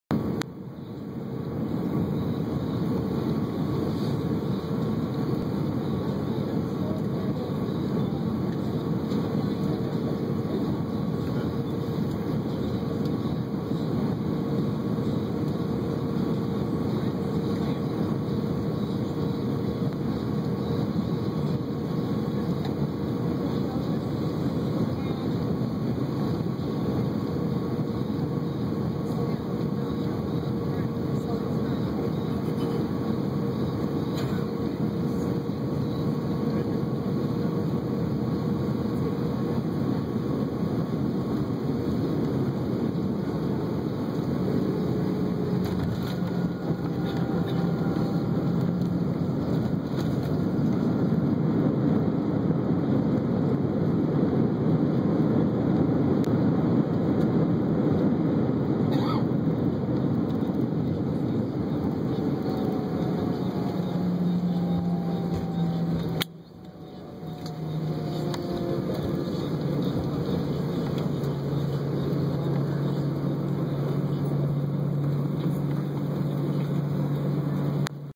Plane Landing in New York City
At LaGuardia Airport.